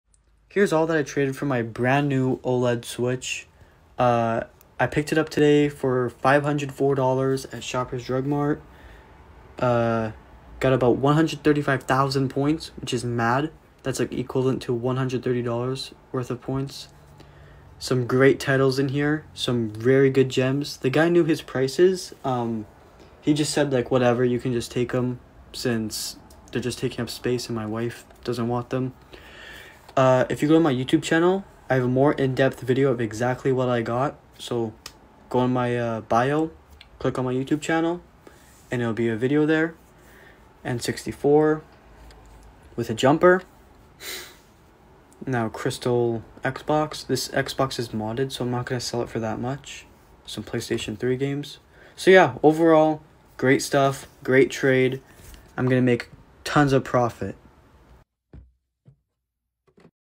My Best Nintendo Switch OLED Trade!
This video was originally an Instagram Reel I uploaded in 2022. This oled trade was my best ever. I don’t think anything will top this trade!!!

The guy did know his prices on his games but he literally didn’t care for them. His wife said they were taking up space and he wanted them off his hands.

Here’s the full video on my YouTube if Interested!